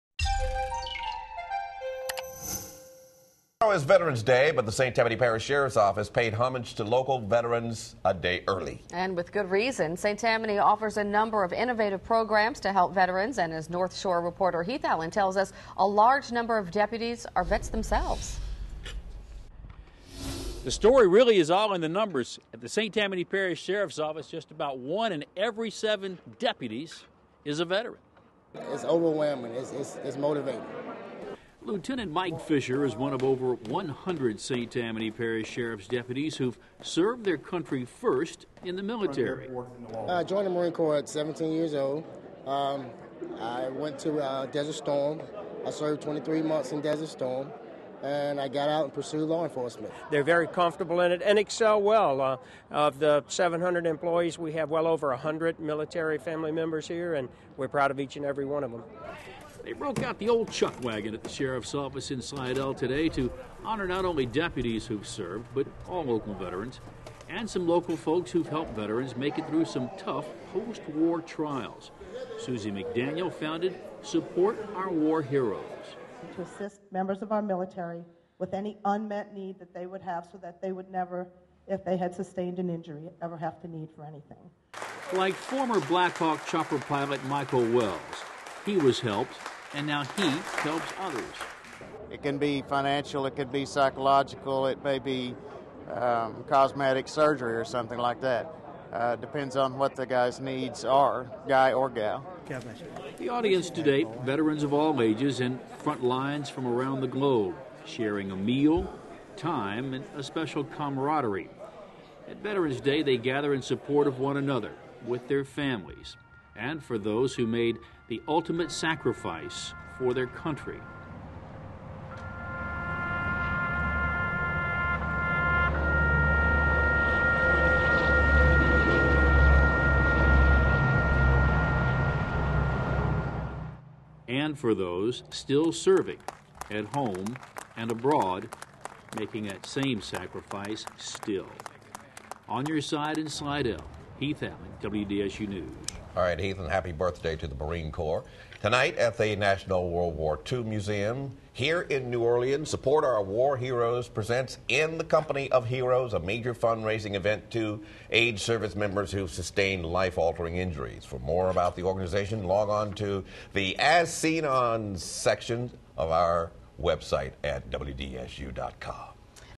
St. Tammany Parish Sheriff's Office Celebrates Veterans
St. Tammany Parish Sheriff's Office paid homage to local vets a day early. The parish offers a number of innovative programs to help veterans.